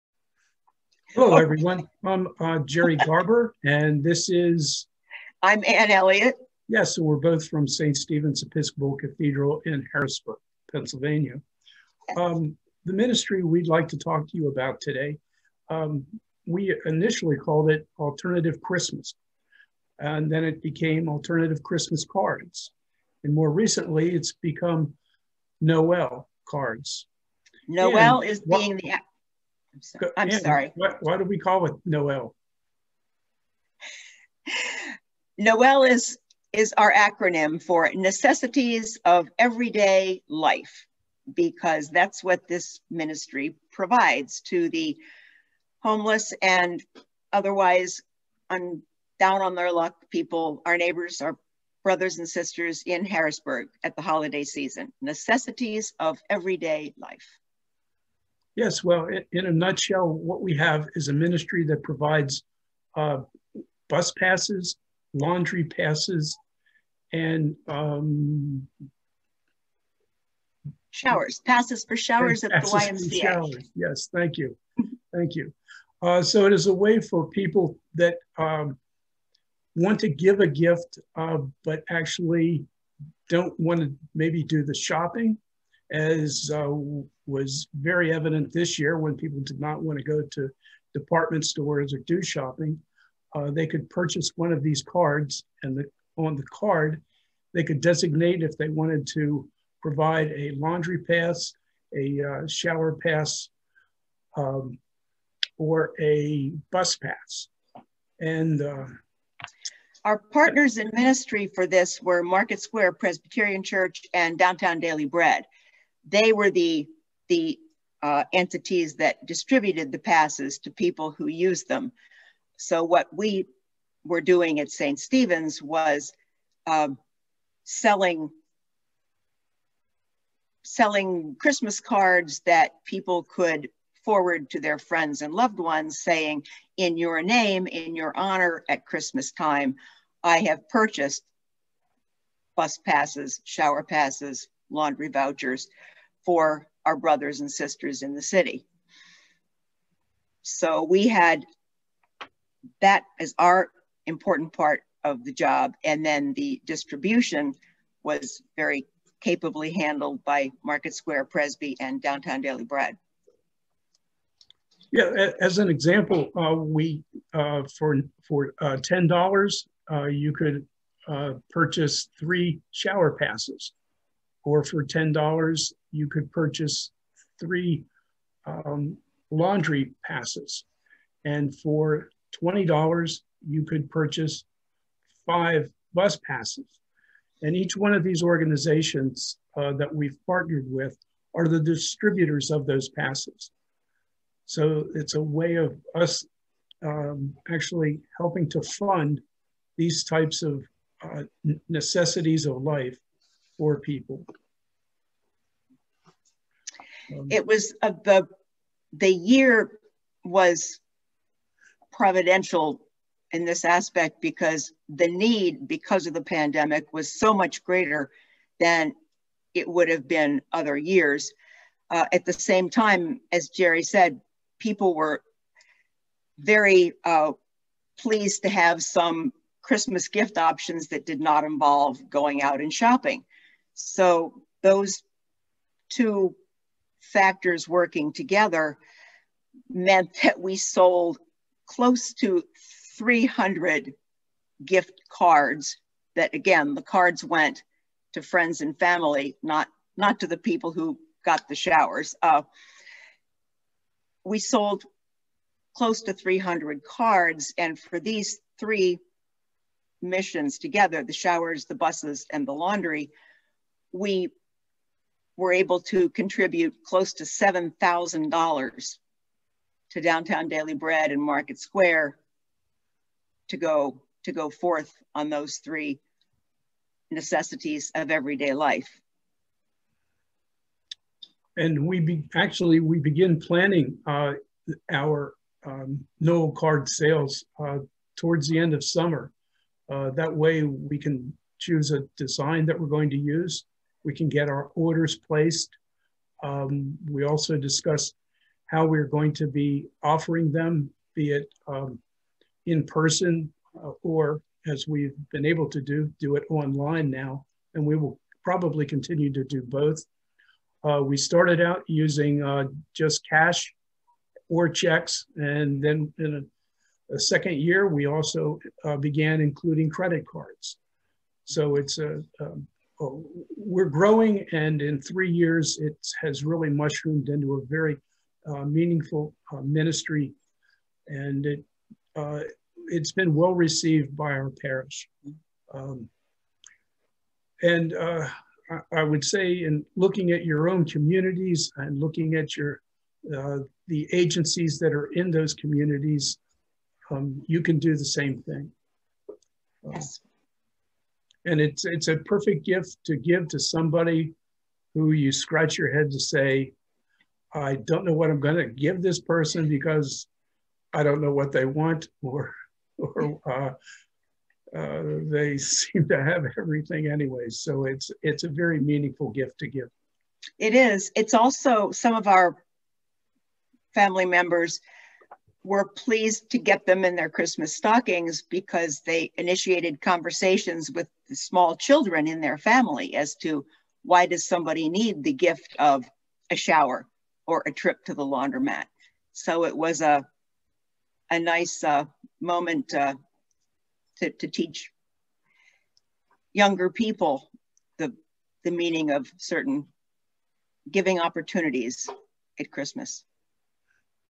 Digital Lenten Calendar: March 2
Gerry Garber and Ann Elliott discuss NOEL ministries from St. Stephen's Cathedral, Harrisburg.